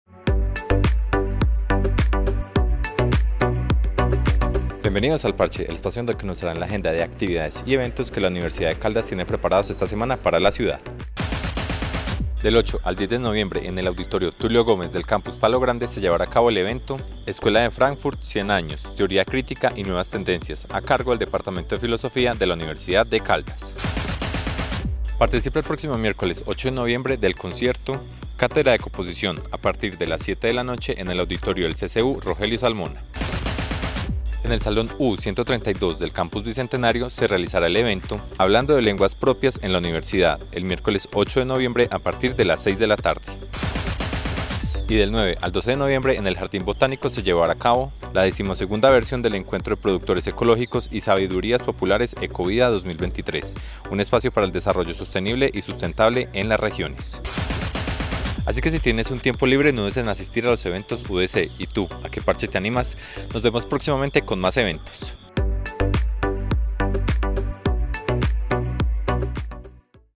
El Parche – Agenda actividades del 7 al 9 de Noviembre
Bienvenidos a El Parche, capsula informativa que contiene la Agenda Cultural, Académica y de eventos desarrollados por la Universidad de Caldas